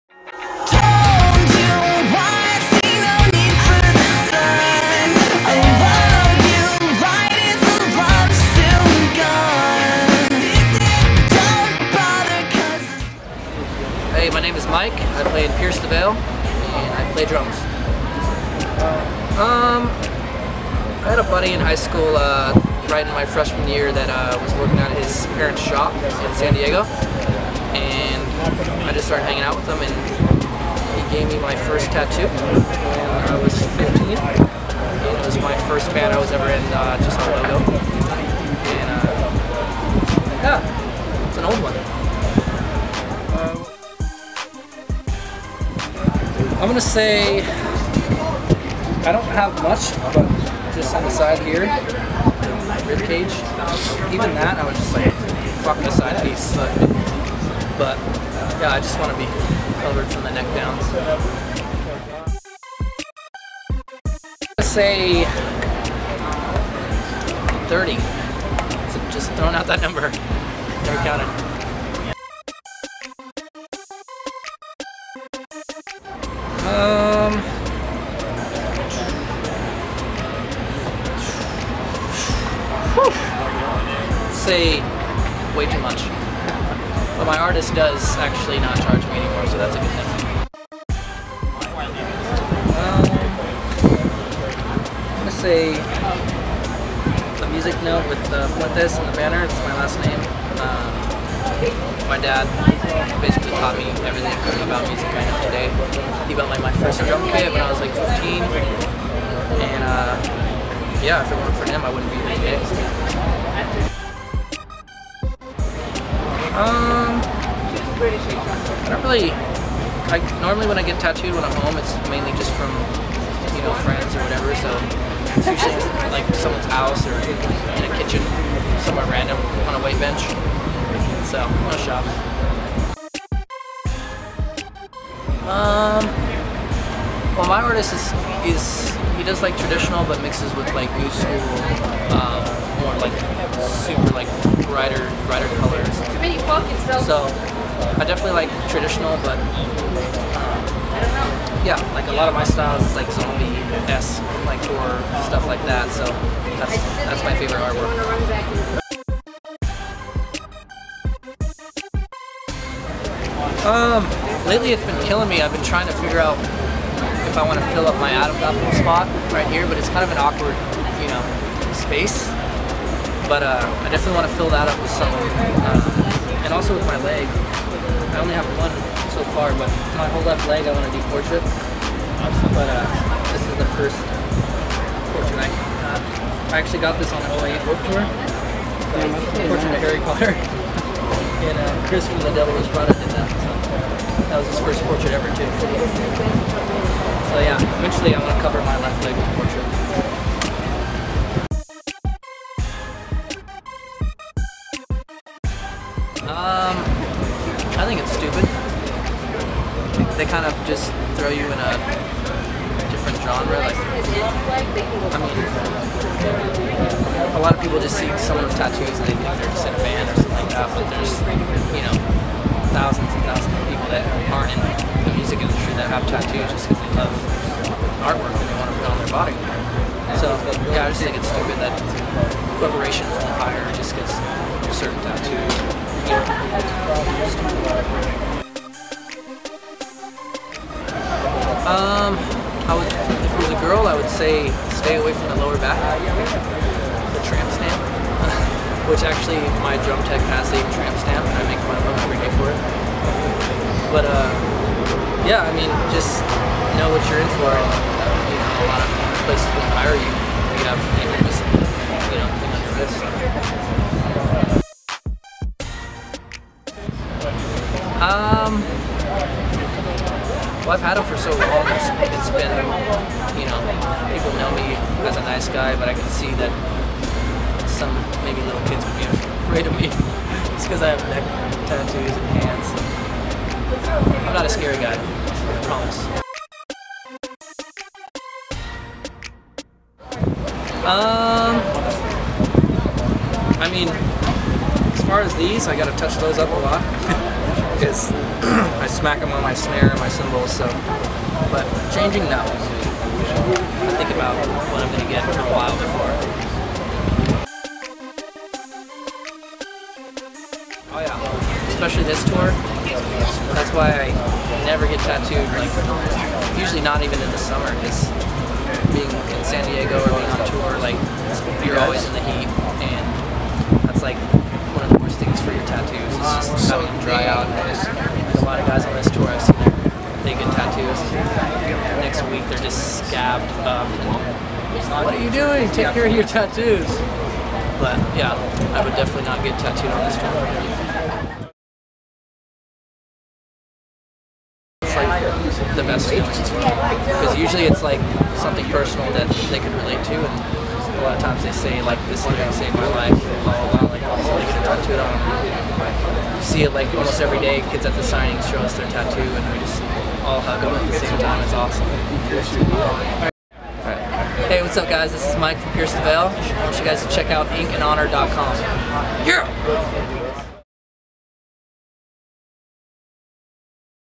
Mike Fuentes Tattoo Interview | Ink And Honor
Tattoo Interview With Mike Fuentes For Ink & Honor
Drummer Of Pierce The Veil